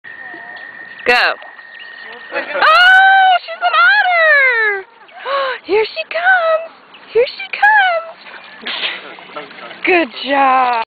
Marley Swimming
Marley Jane in her first swim (that we've seen) at Governor Dodge State Park, WI: May 08